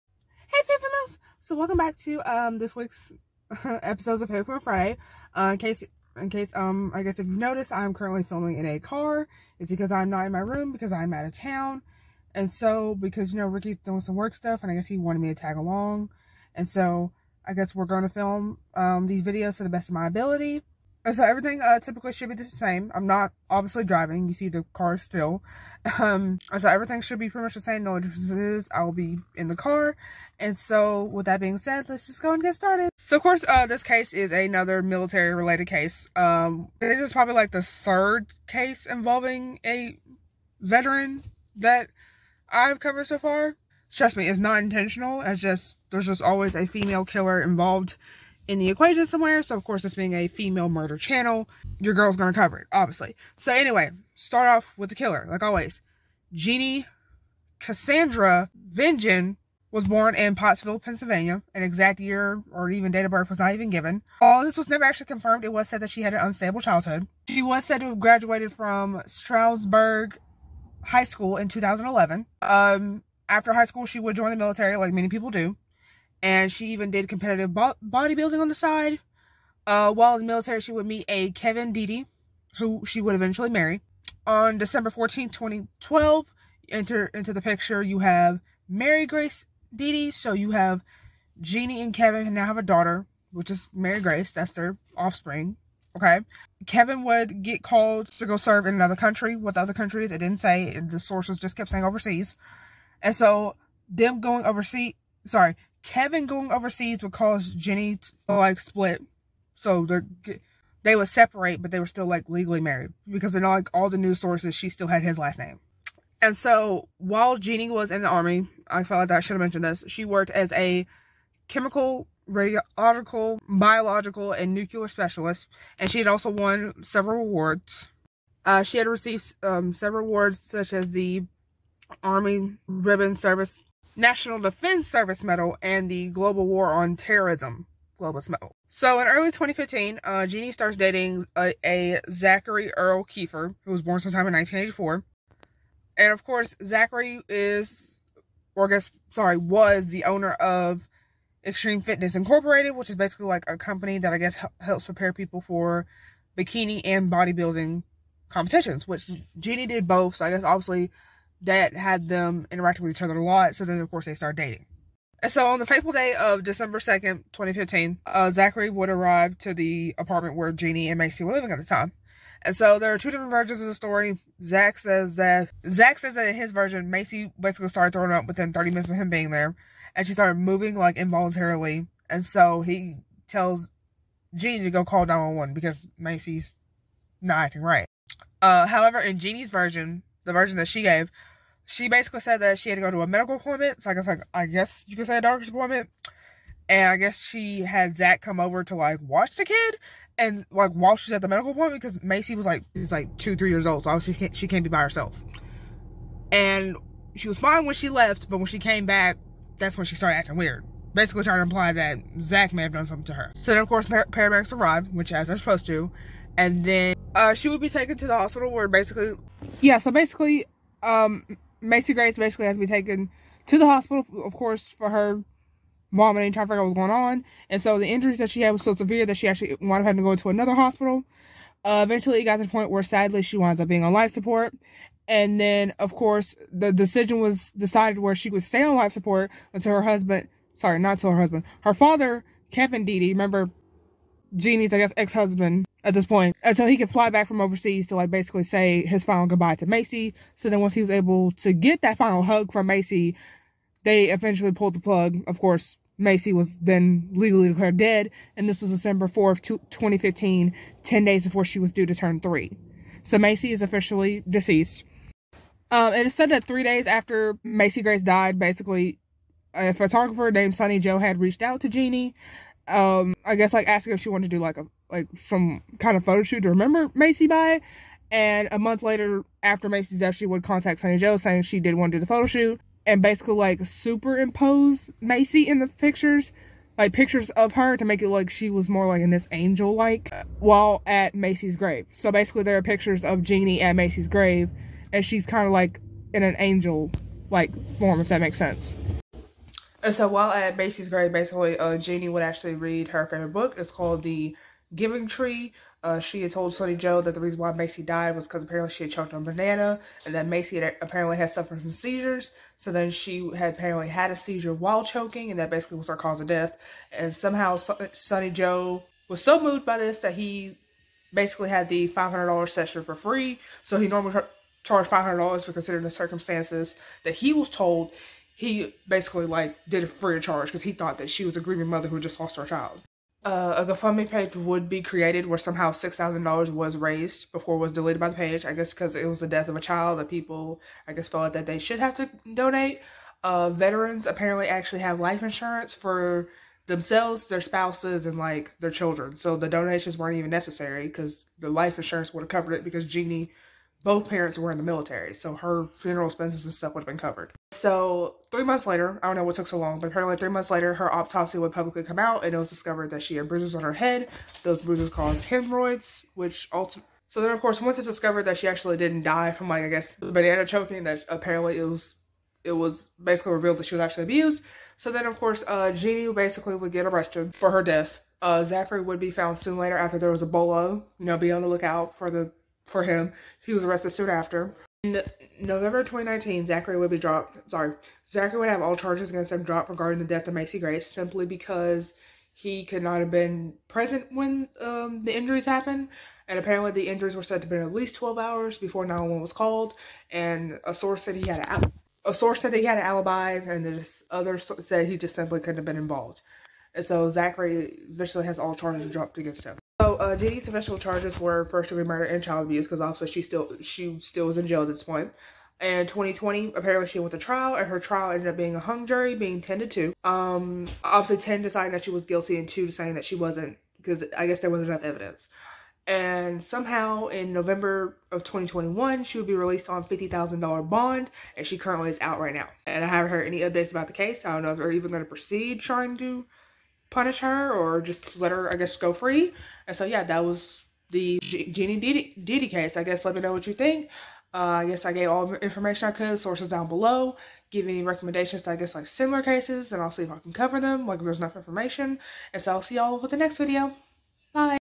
Dishonorable Ditty (Murder of Macy Grace Ditty)
Hey Fatal Females! 👋🏾 👋🏾 👋🏾

Just a heads up, it will be one case this week. Been working pretty much every day & I wasn't able to record this until the day of uploading. that's why it seems so rushed & why I'm in different settings. Bare with me & try to enjoy. :)

Sources 💫 💫 💫